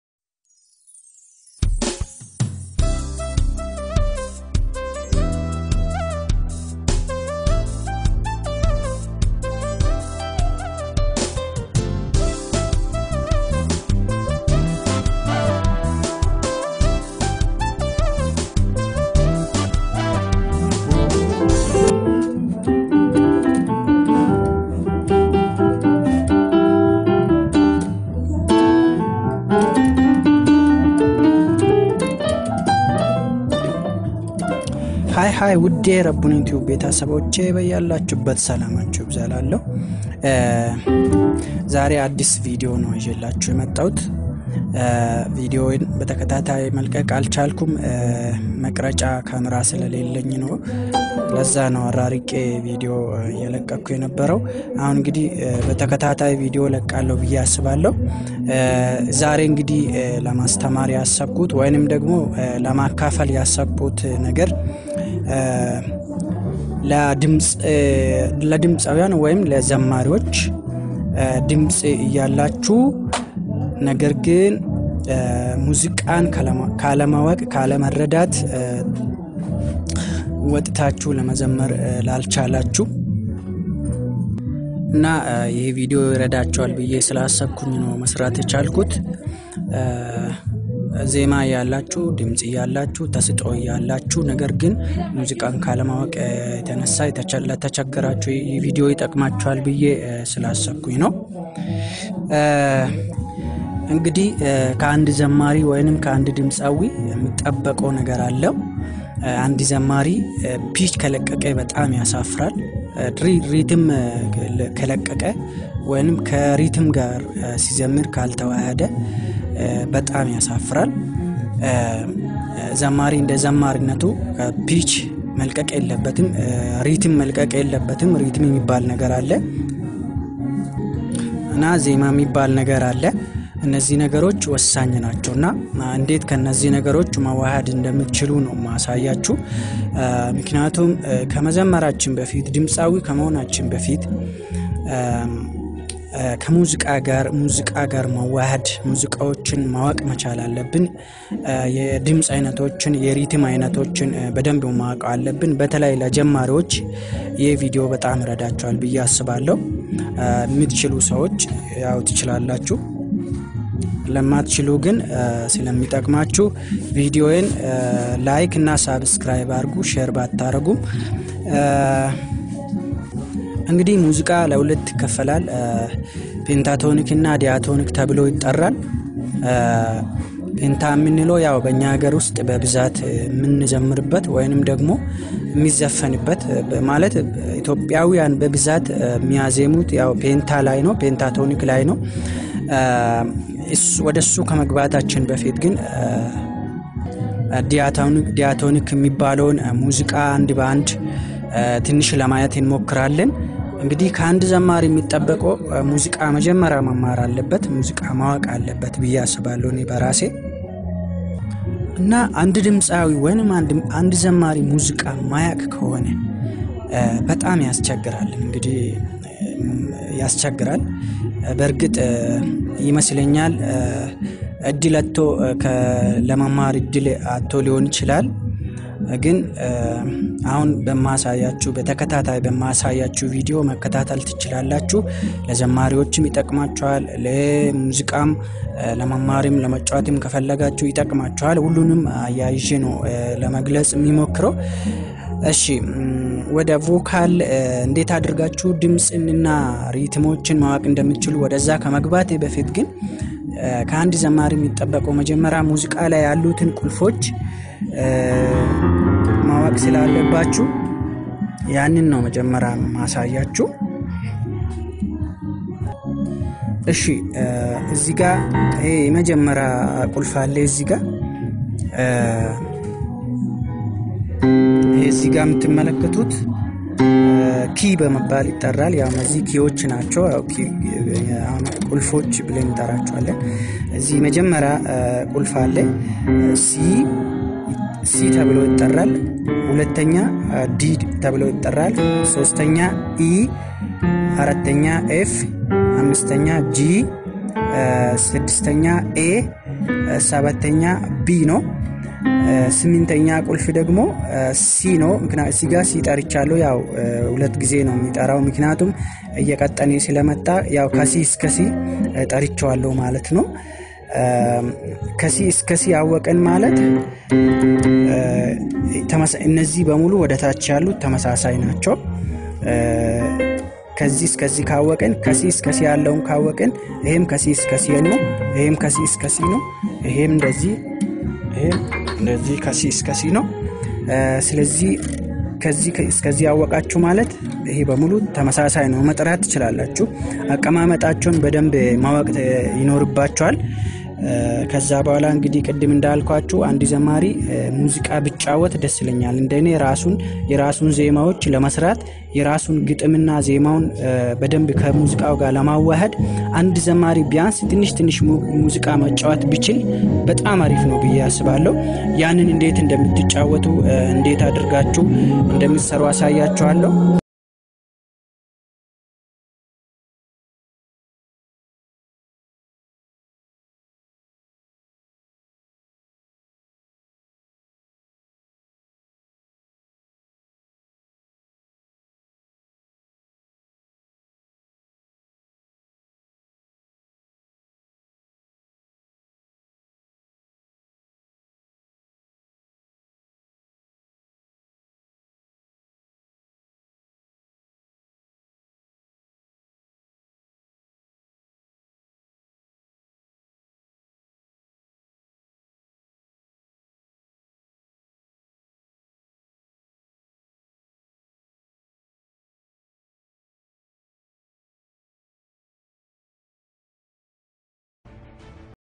የሙዝቃ ትምህርት ለጀማርዎች
ይህ ትምህርት ለዘማሪያን (ለድምፃዊያን በተለይም የሪትም እና የቅኚት ችግር ለለባቸው በሙሉይረዳቸወል